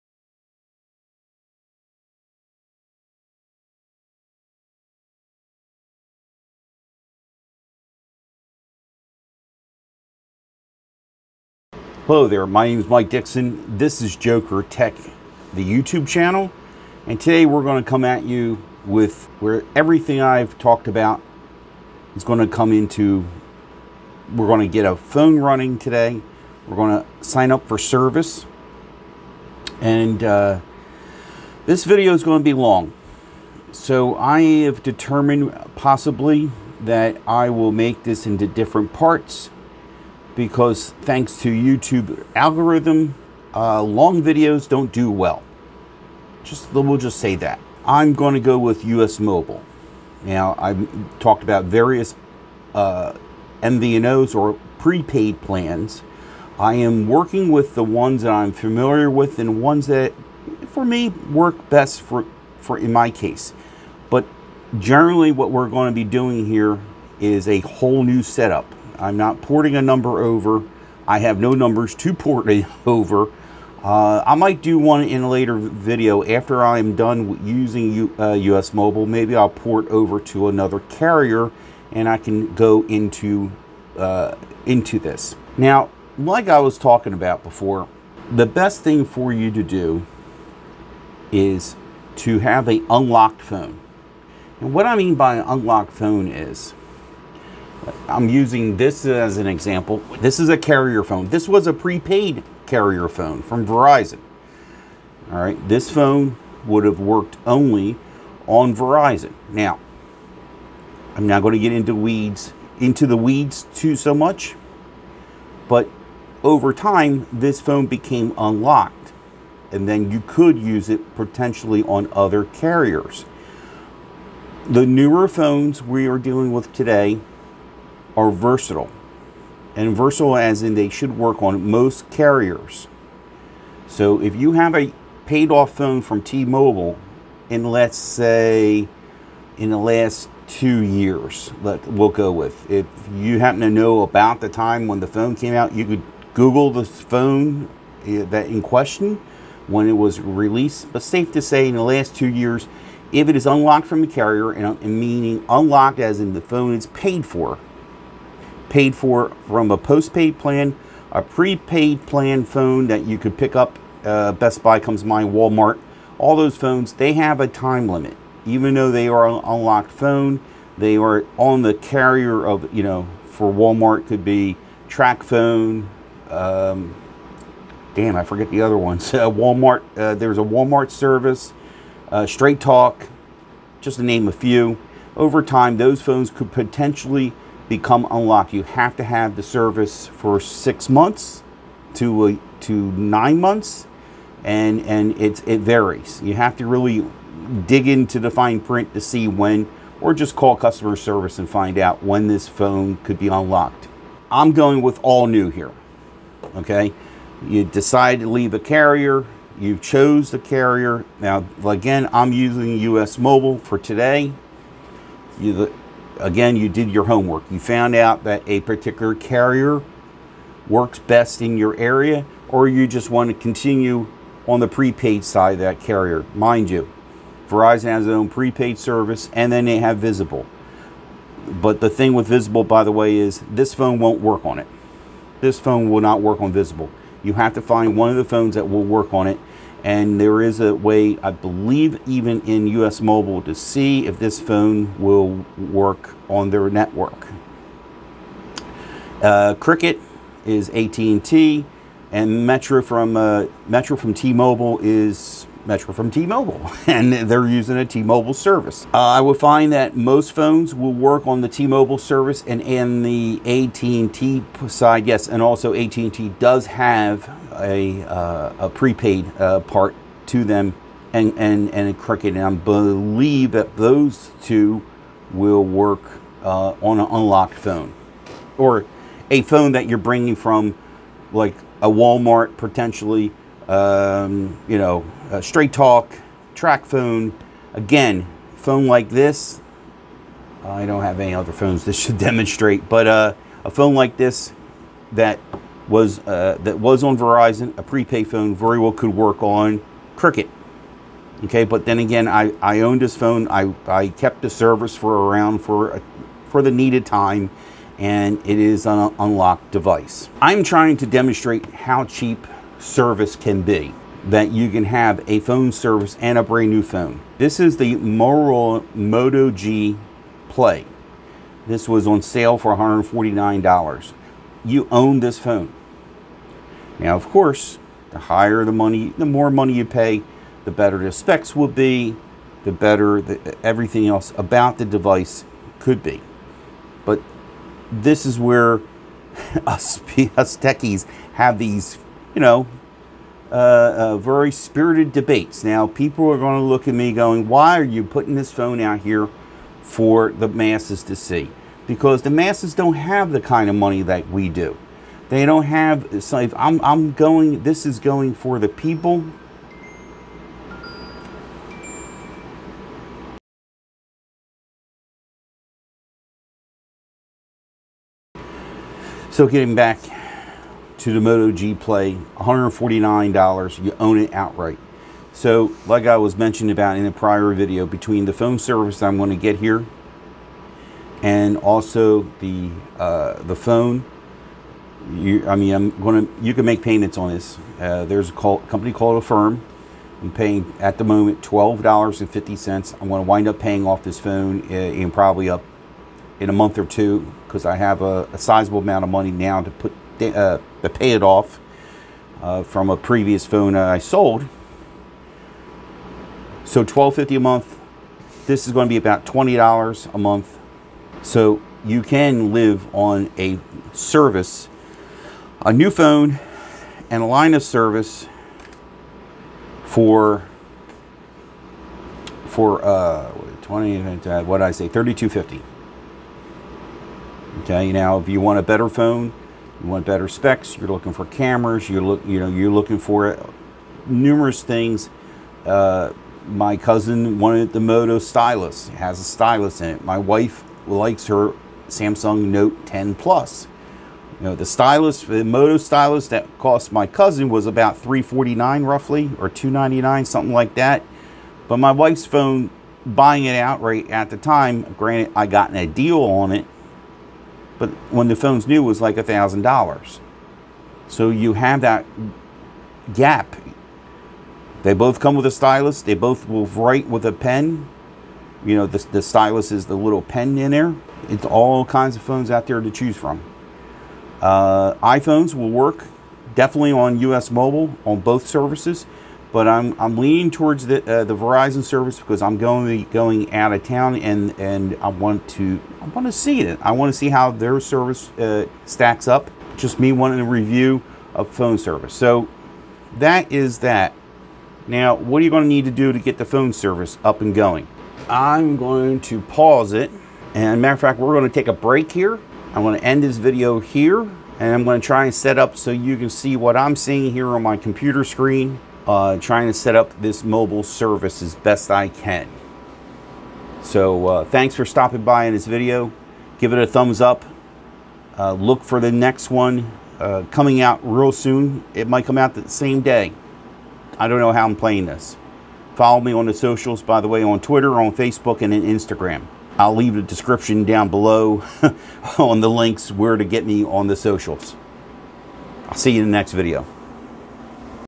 Setting up your new phone plan. Part 1
Now we get down to business.  Let's set up that new plan and save some money !
A lot to go over. Don't want to miss anything. Stick around for the other parts.